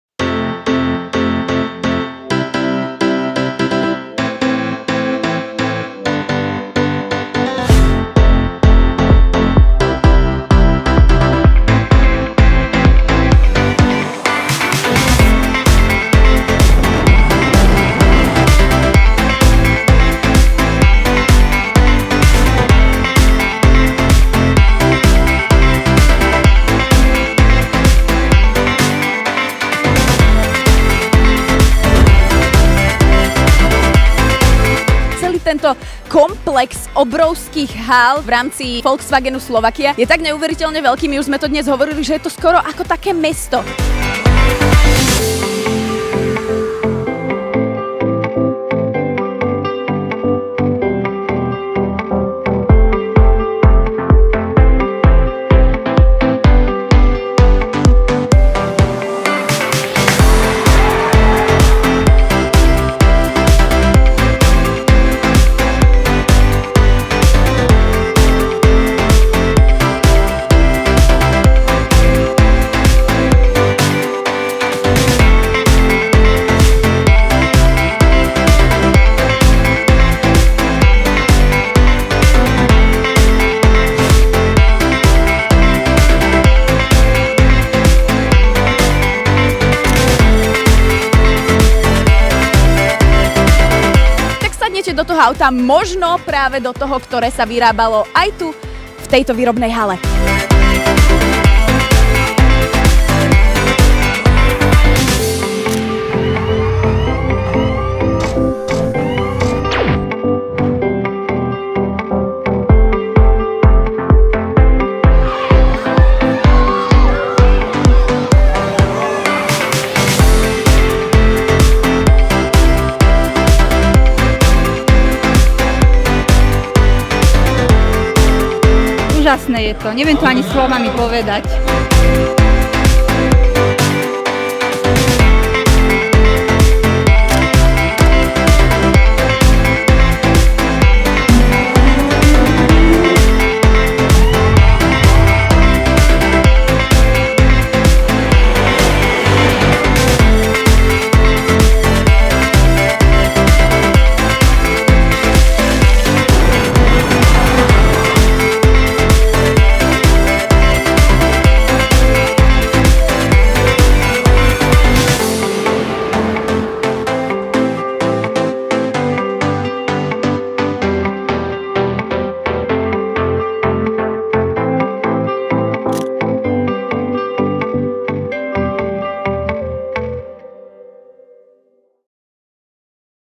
Deň otvorených dverí 2016 Volkswagen Slovakia | Aftermovie
Oslávili sme 25-ku! Ako? So všetkým, čo k tomu patrí! Na Deň otvorených dverí sme do Volkswagen Slovakia pozvali našich zamestnancov, ich rodiny, známych, širokú verejnosť a oslavy mohli začať. Náš závod privítal neuveriteľných 30 000 ľudí, ktorých celý deň zabávali kapely ako IMT Smile, Tublatanka, Majk Spirit, Celeste Buckingham a ďalší. Kopec atrakcií, dobrého jedla a zábavy len potvrdili, že Volkswagen Slovakia možno pokladať za jednu veľkú rodinu, kde má každý svoje miesto. Veľká vďaka tým, ktorí s prípravami pomáhali, ale aj tým, čo sa zúčastnili. Vidíme sa o rok!